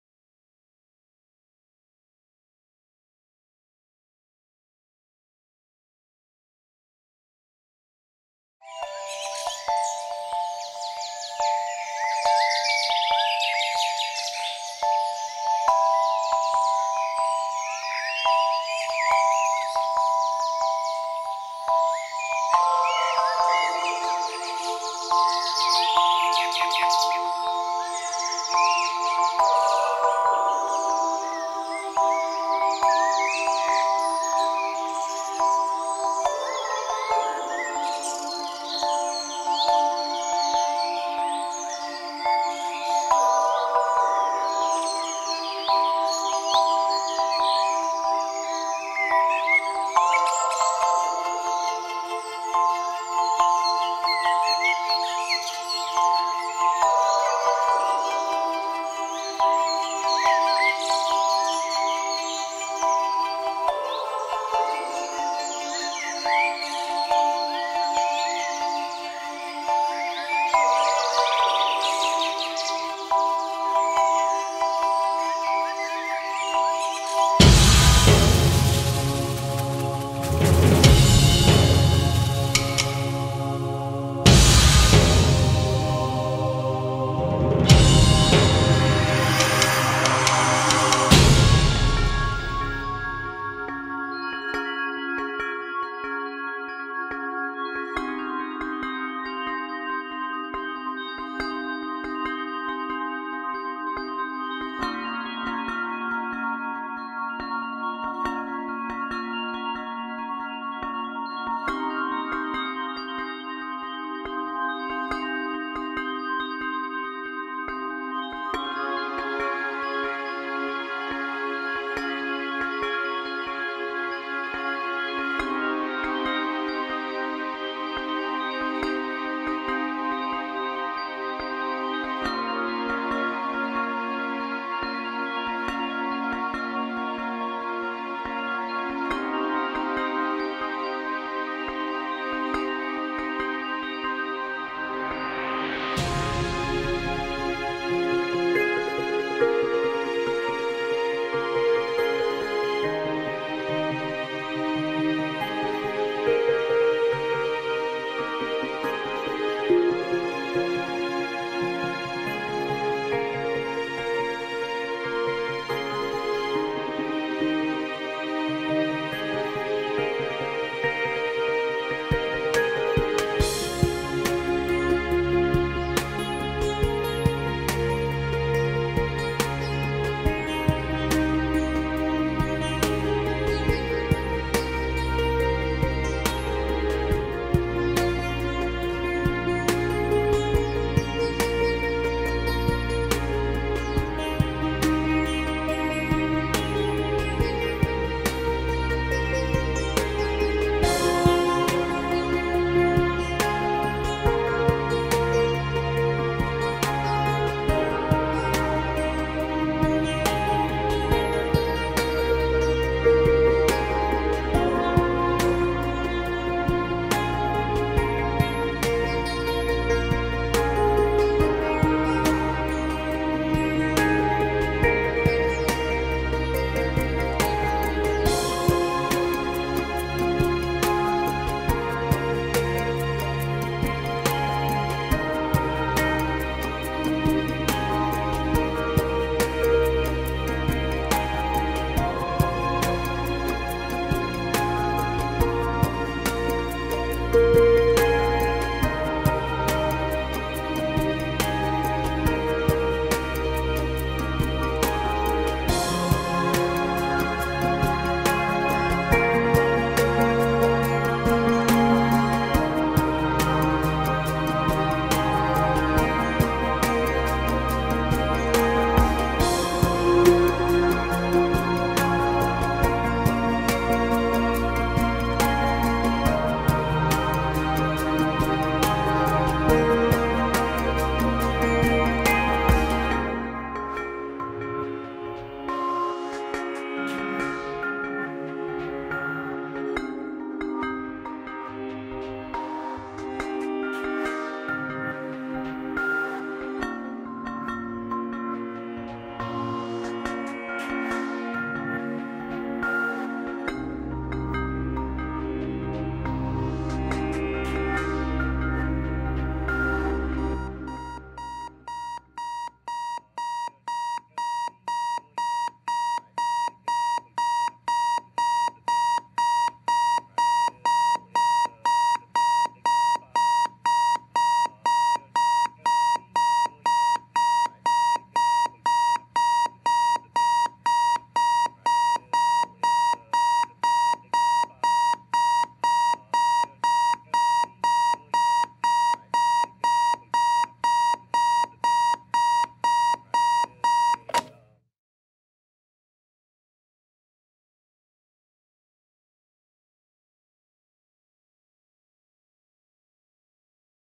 The Dream Center -part 1
Teodora Ionescu video  / interactive installation
- cube made from several layers of transparent sheets. The multiple layers allows the projection to take on a holographic effect.
 The first part of the installation  consists of  a  6 minutes movie. The public is invited to experience  the state of meditation, sleep, dream and awaking.  They can experience this sitting down ( chairs or fatboy) or moving all around. Relation between time,space and reality is diluted.